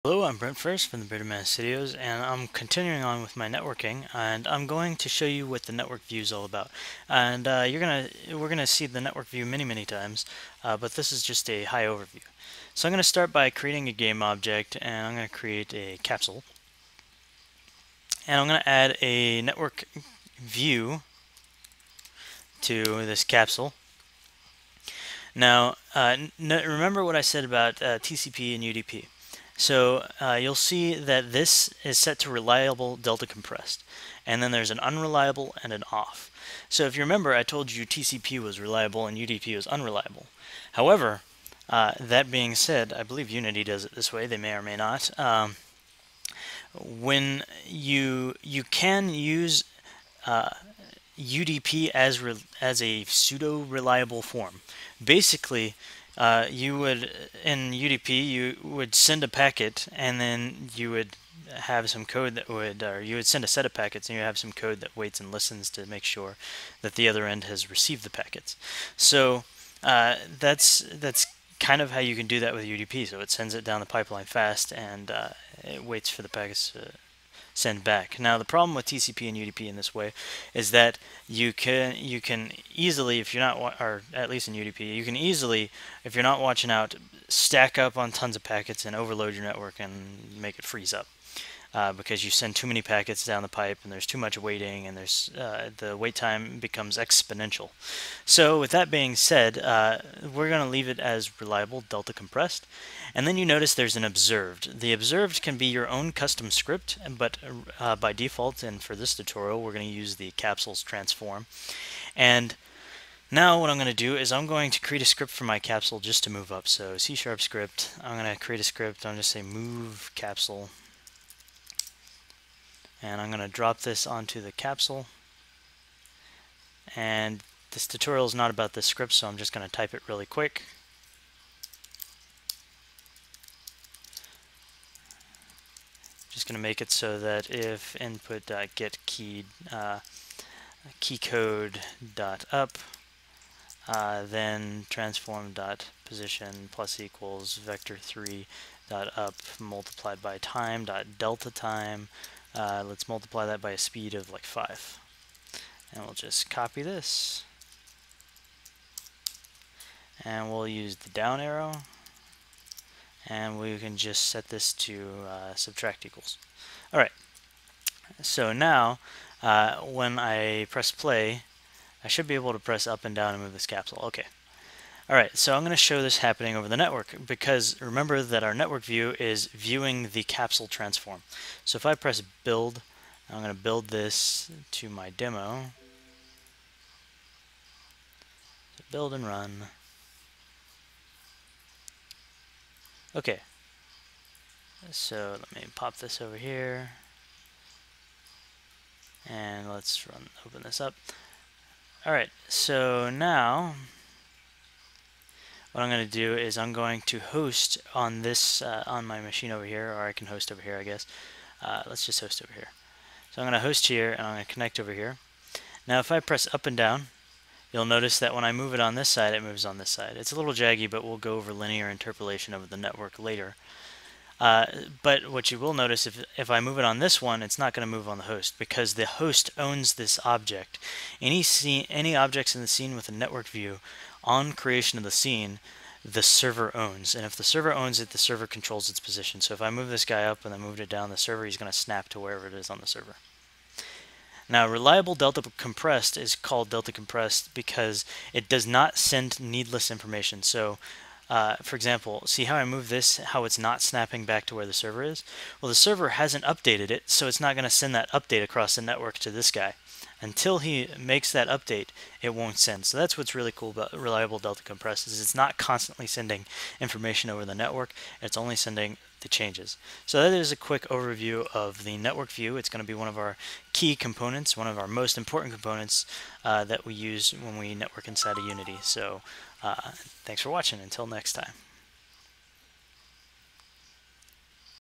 Unity3D Networking 3: The Network View
This video goes over the built in component in Unity3d called the "Network View".